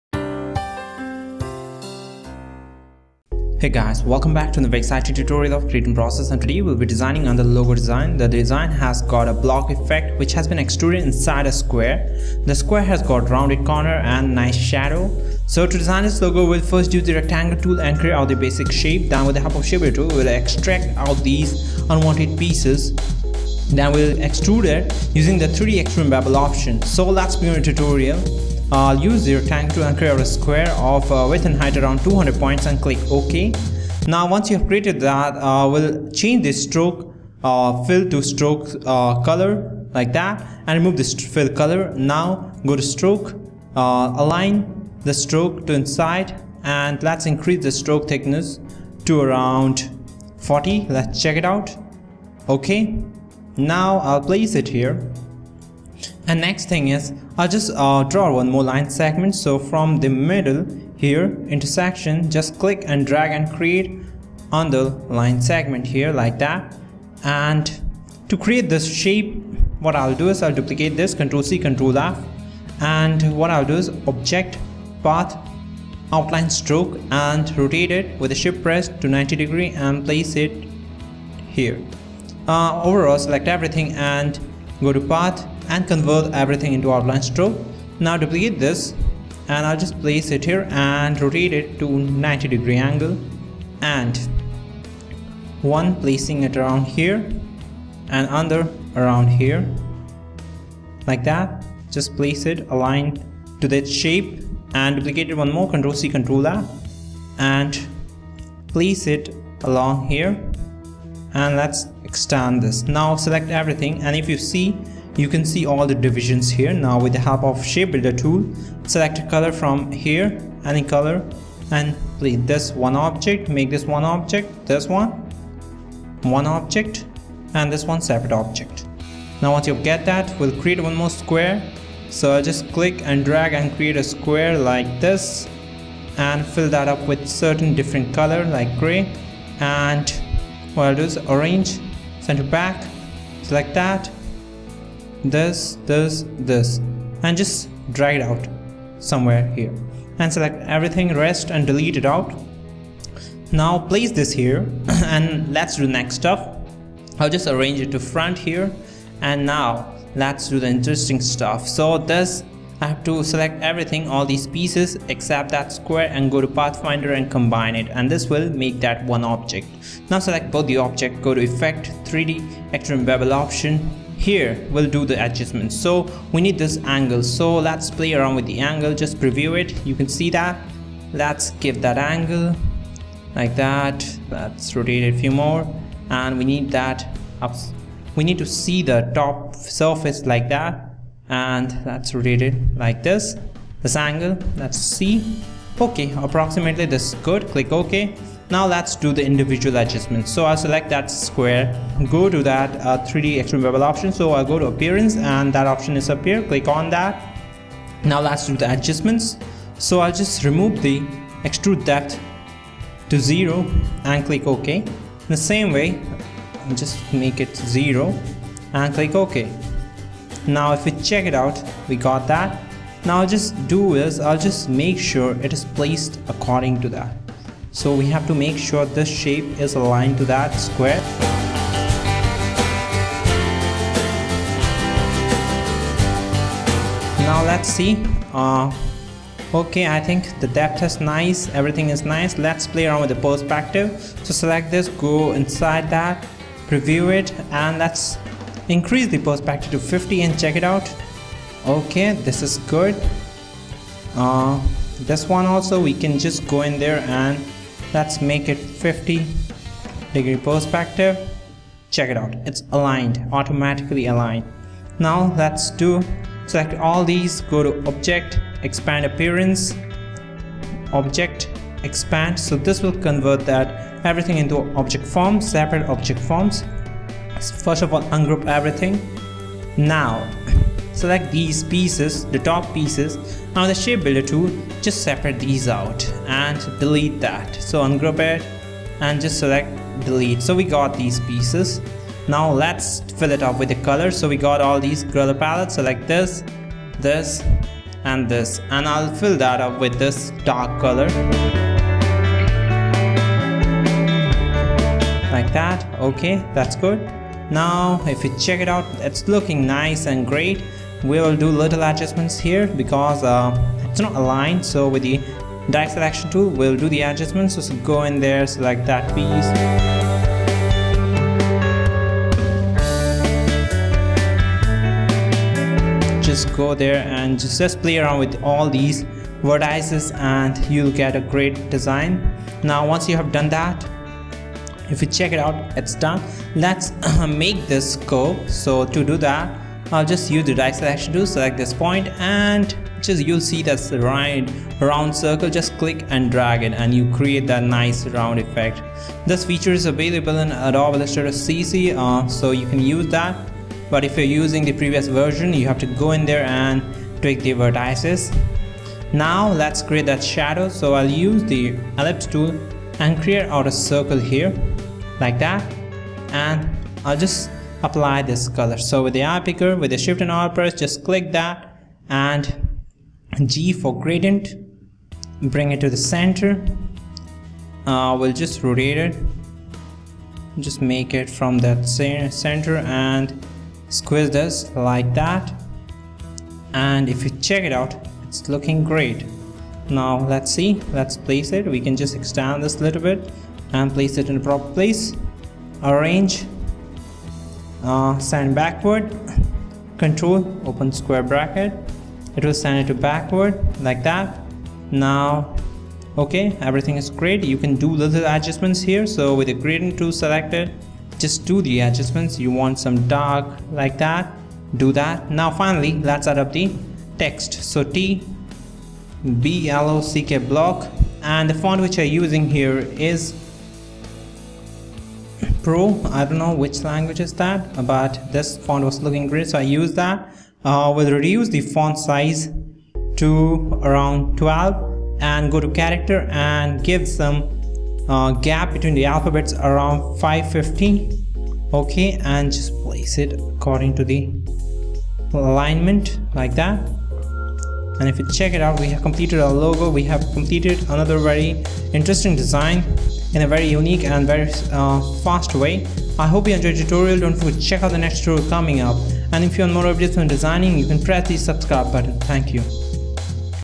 Professional Logo Design - Adobe Illustrator cc (BLOCK)
Here is another Logo Design Tutorial. The design has got block effect, which has been extruded inside the square. The square have rounded corner and nice shadow.
To design this logo we will first use the rectangle tool and create out the basic shape, then with the help of shape builder tool we will extract out the unwanted pieces. Then extrude it using the 3d extrude and bevel option. So check out the tutorial for further details
If you want to Know more about Designing,

▲Gumroad Store!

▲Creatnprocess Channel!